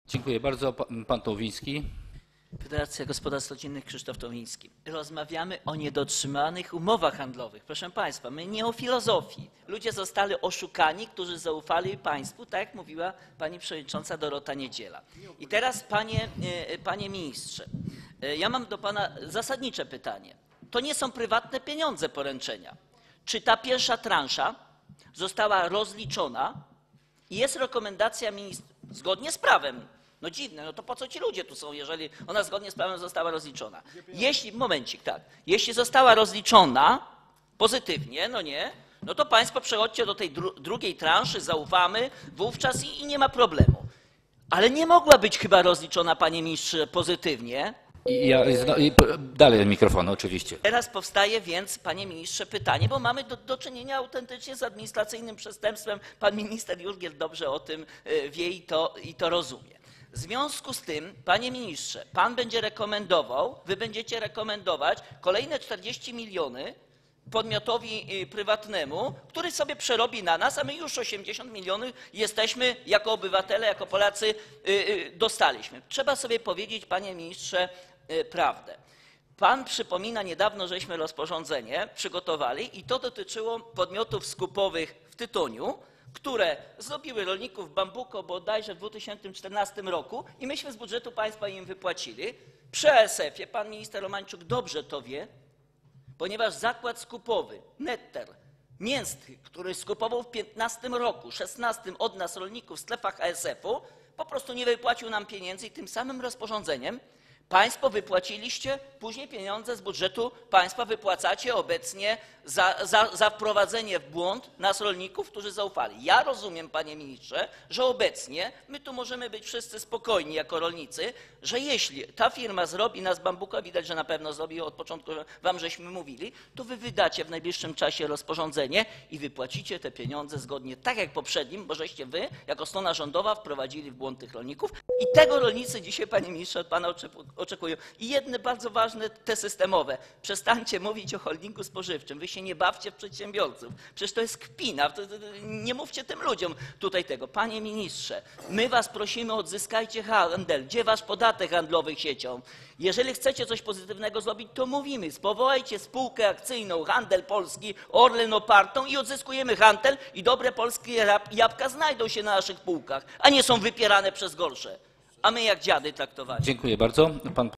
Co łączy Ministerstwo Rolnictwa z Grenlandią? -" ESKIMOS"
Wysoki publiczny rachunek za interwencję na rynku jabłek dokonany przez Ministra Ardanowskiego.
Jako podatnicy rezerwujmy 80 mln złotych z budżetu państwa na wypłatę odszkodowań oszukanym przez Spółkę "Krzak" , przedsiębiorstwom skupowym i producentom jabłek. Ministerialny projekt Spożywczego Holdingu ma zadatki na jeszcze większą "przewałkę" niż z "Eskimosem".
Zalecany kierunek - to odzyskanie wpływu na handel poprzez budowę Polskiej Sieci Handlowej o rozpoznawalnej marce w formie Spółki Akcyjnej.

Krzysztof Tołwiński - Polski Polityk.
Sejmowa Komisja Rolnictwa , posiedzenie w dniu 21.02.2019 r.